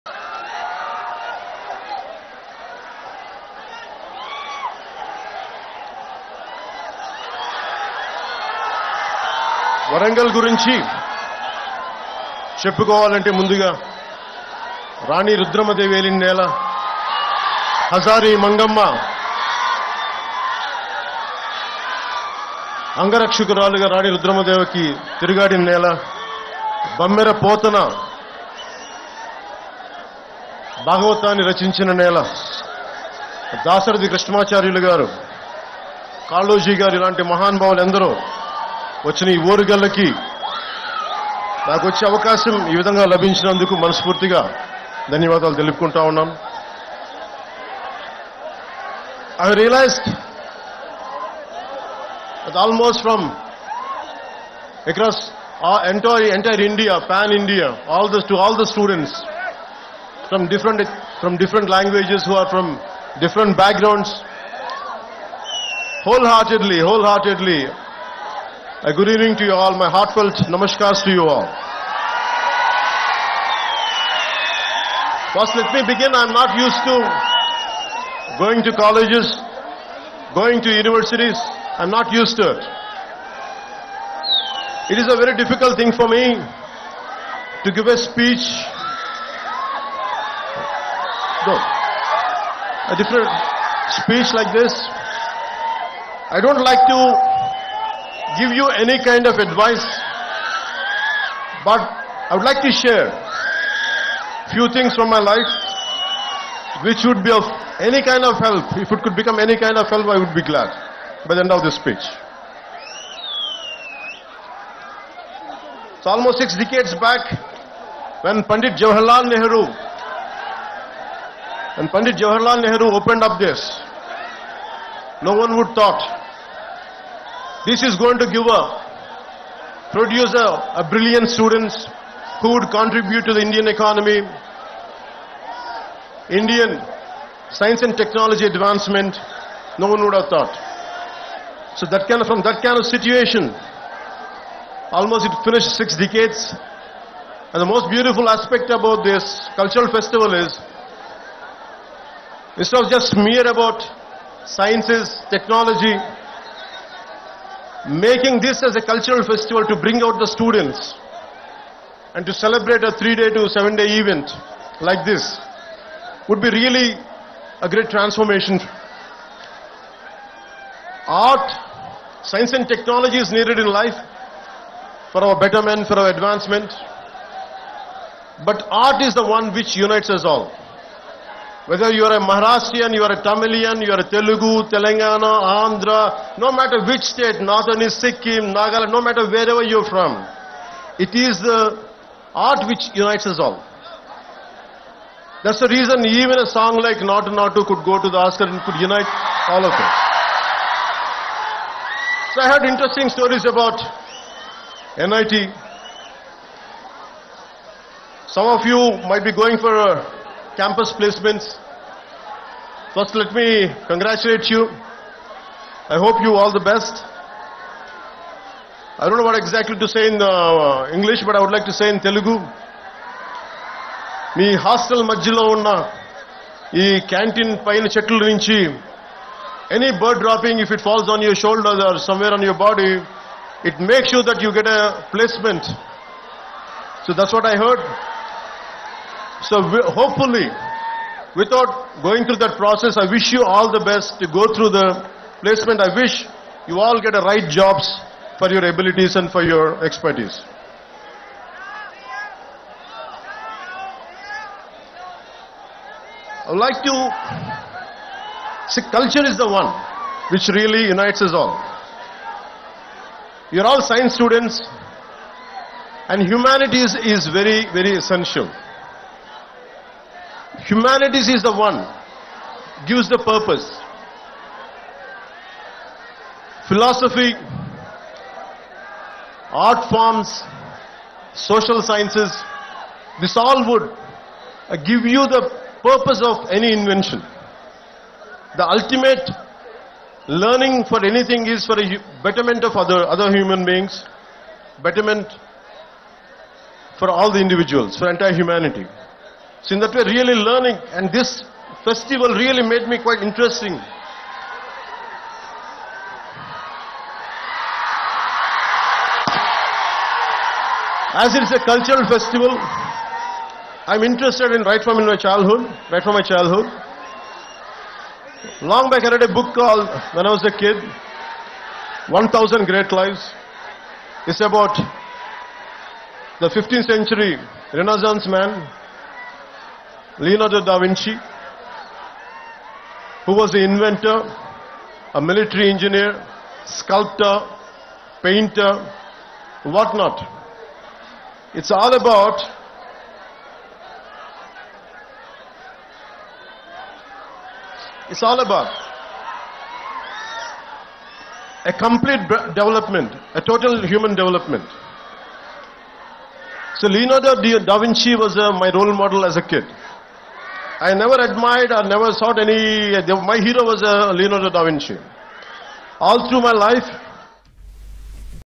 Springspree 2023 : Janasena Chief Pawan Kalyan Visits Warangal NIT | V6 News
లీకుల్లో రాజకీయం